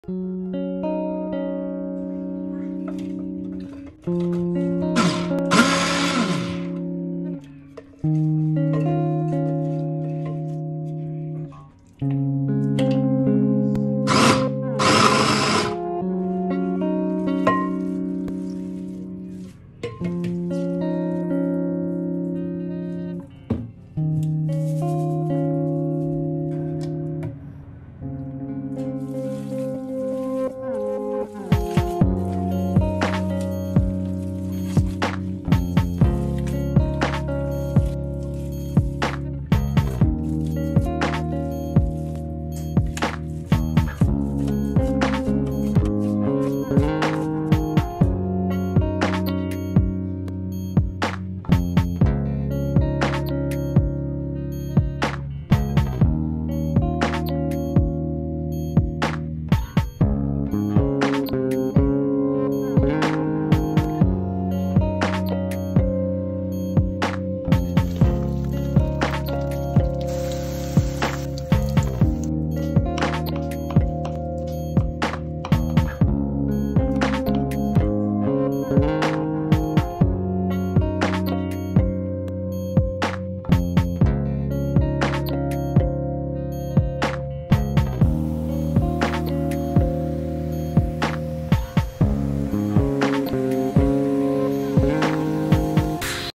كباب دجاج بالتبسي
المقادير:
لكل كيلو صدر دجاج نصف كيلو افخاذ دجاج منزوعة العظم والجلد
فلفل احمر بارد
فلفل اخضر
3 قطع بصل وكل هذه الخضار مقطعة ومفرومة ناعمه وبعدها تخلط مع الدجاج المفروم
البهارات ( فلفل اسود وفلفل احمر باودر وملح )
ربع كوب زيت يضاف على الوجه قبل الشواء

وبالعااااافية عليكم